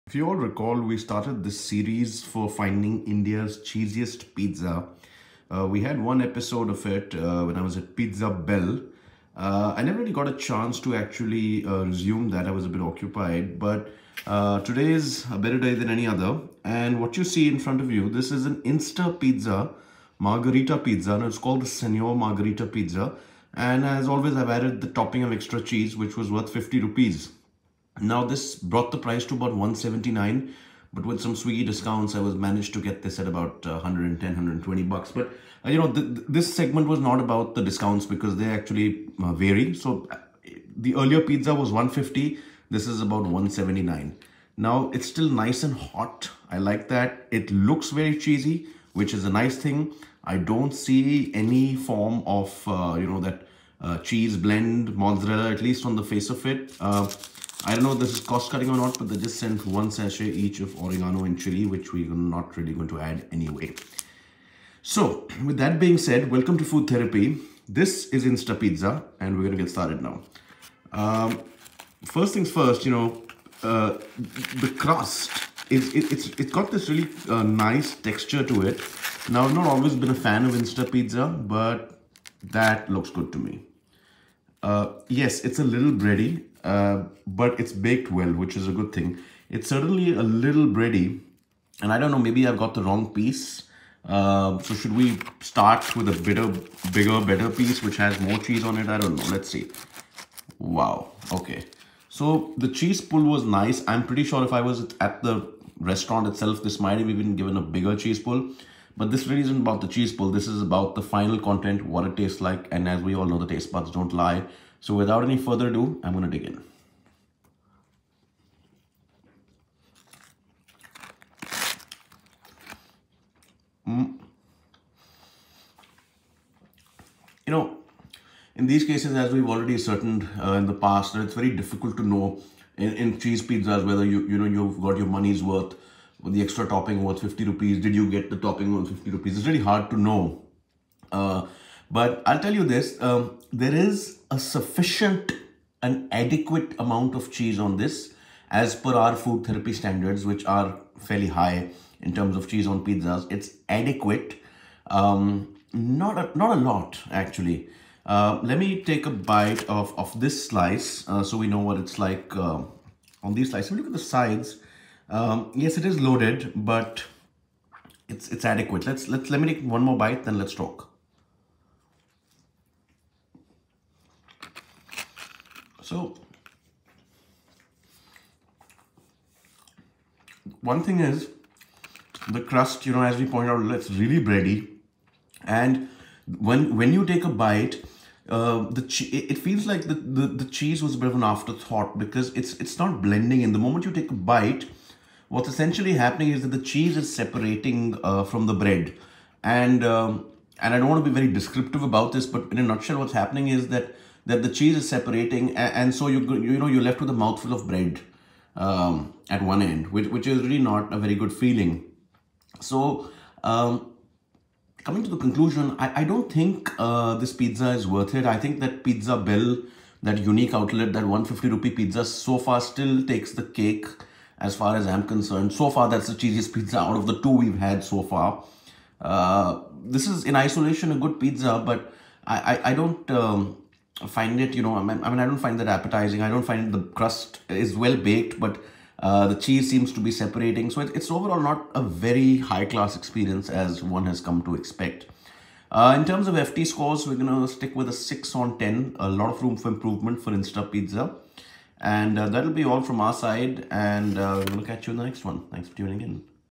Finding India’s Cheesiest Pizza Feat Insta Pizza S1 E2
We continue the previous series of finding India’s cheesiest pizza, Episode 2 features Insta Pizza, with their margherita pizza with extra cheese, it retails at Rs. 180.

Stuffed Crust cheese max : https://
youtu.be/_5QNu0iLV30